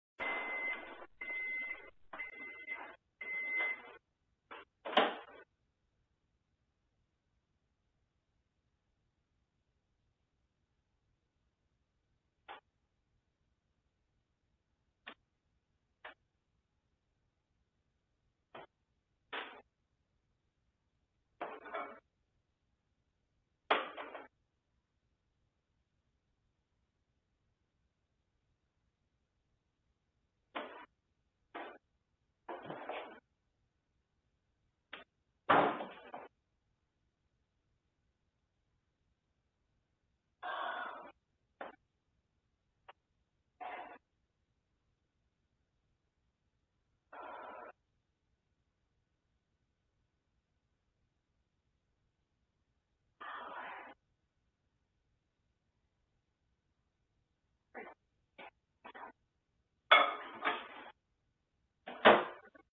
INDIANA MOM BANGS HEAD ON MICROWAVE DOOR OUCH!
This is basically a typical minute in Liberty's life. Simply using the microwave nearly (Or quite possibly) has given her a mild concussion. *Sound on for the full effect.